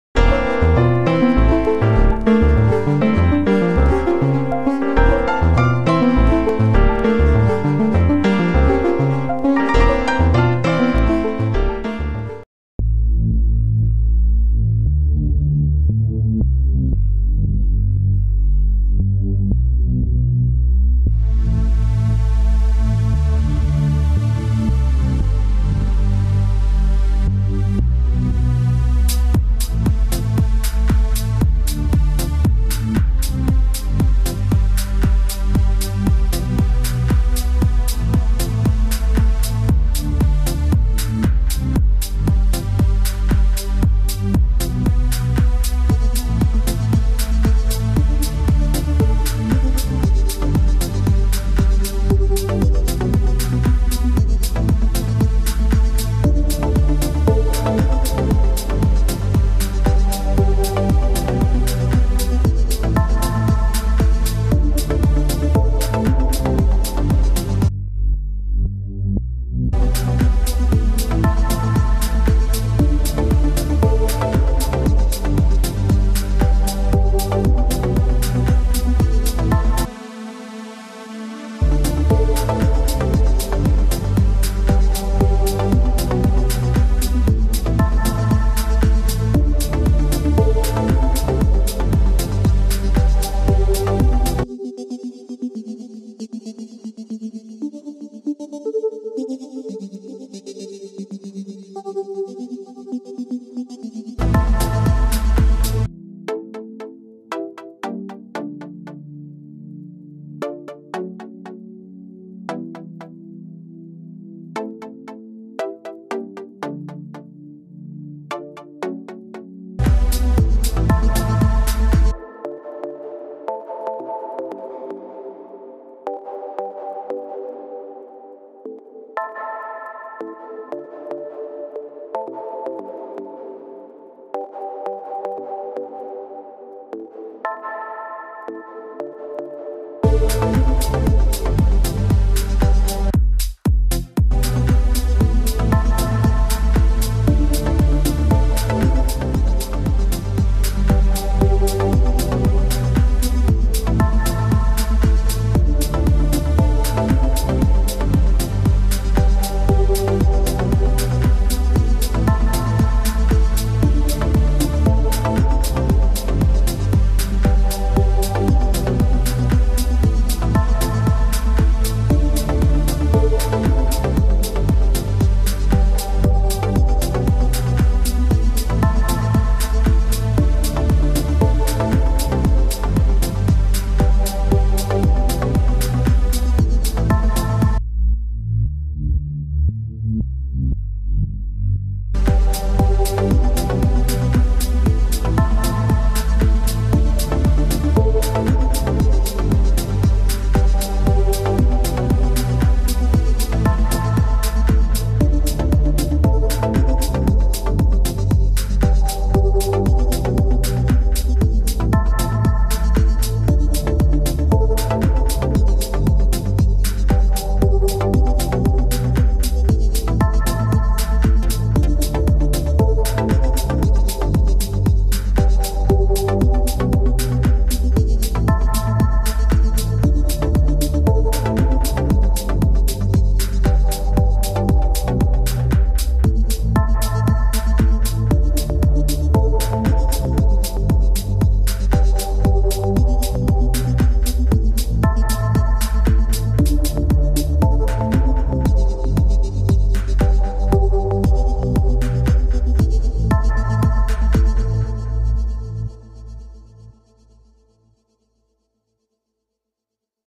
Kv331 Audio Synthmaster 2 vsti + Reason 9.5
Nuevamente les dejamos el proyecto en manos de ustedes para que lo terminen o modifiquen a su gusto abajo le dejamos el enlace de descarga, nota no fomentamos la piratería tanto Reason como Synthmaster tienen que tenerlos por cuenta de ustedes.

Por favor si les pareció útil son bienvenidos a subscribirse al canal, recuerden que estarán apoyando a los futuros vídeos.